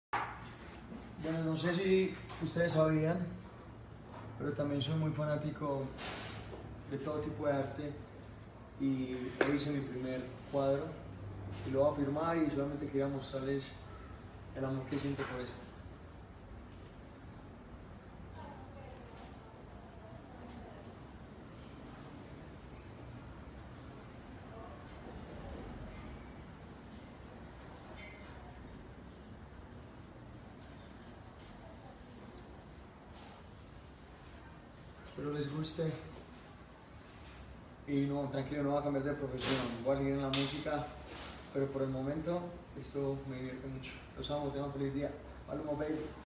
Maluma made first painting | Maluma 2019
Maluma made first painting. It is for sale